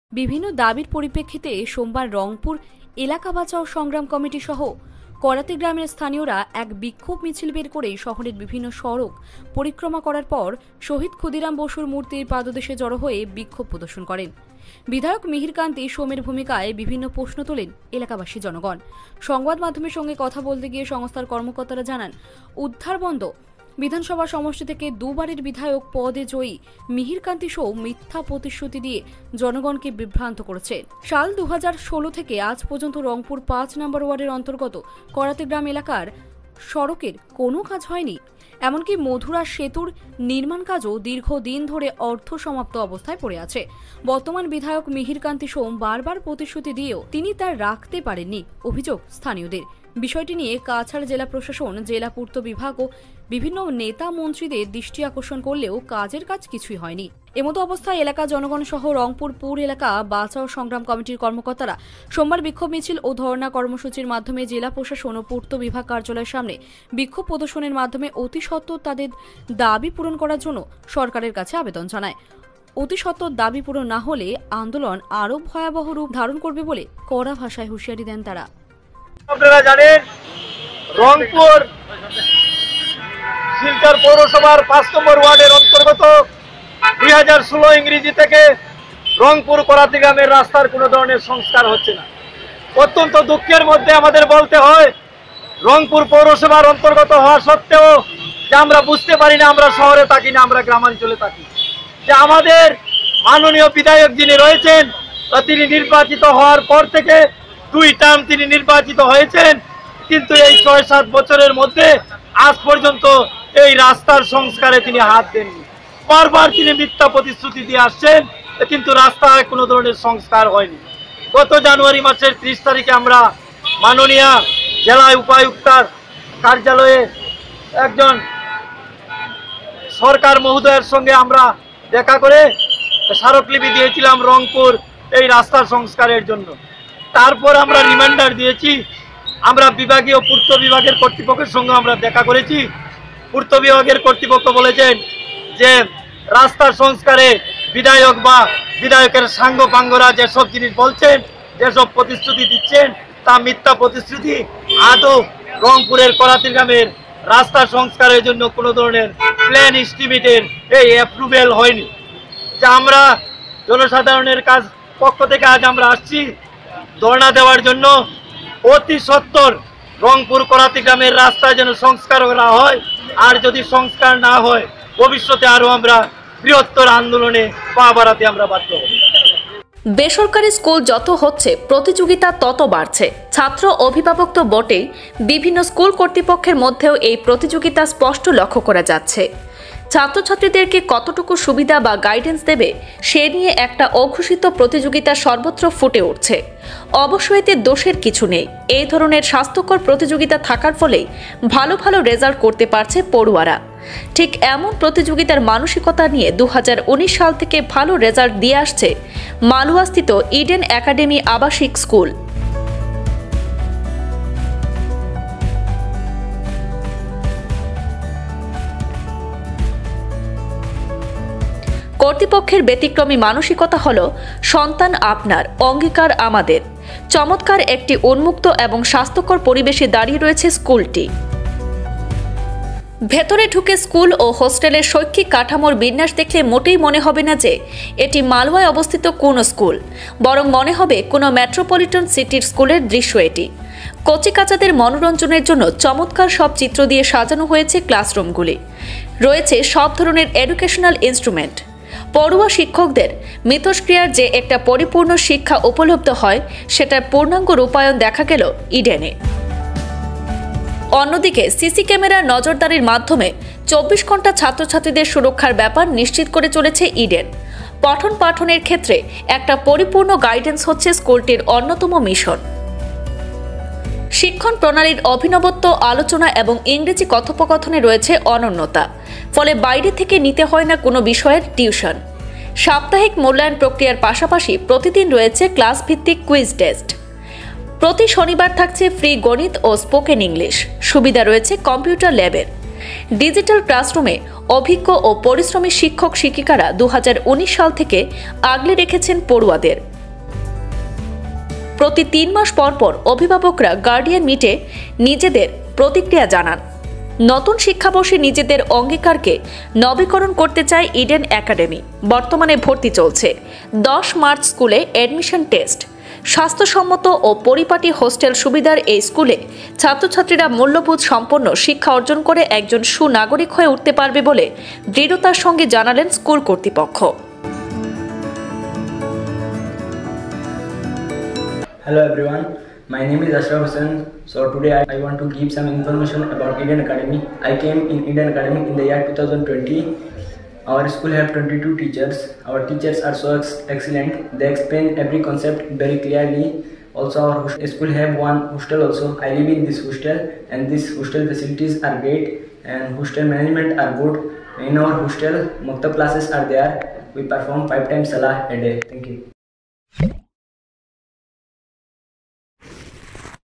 শিলচর রংপুর করাতিগ্রাম এলাকার সড়ক সংস্কার ও অর্ধসমাপ্ত মধুরাসেতু নির্মাণের দাবিতে বিক্ষোভ মিছিল
শিলচর রংপুর করাতিগ্রাম এলাকার সড়ক সংস্কার ও  অর্ধসমাপ্ত মধুরাসেতু নির্মাণের দাবিতে বিক্ষোভ মিছিল ও ধর্ণা প্রদর্শন  এলাকাবাসির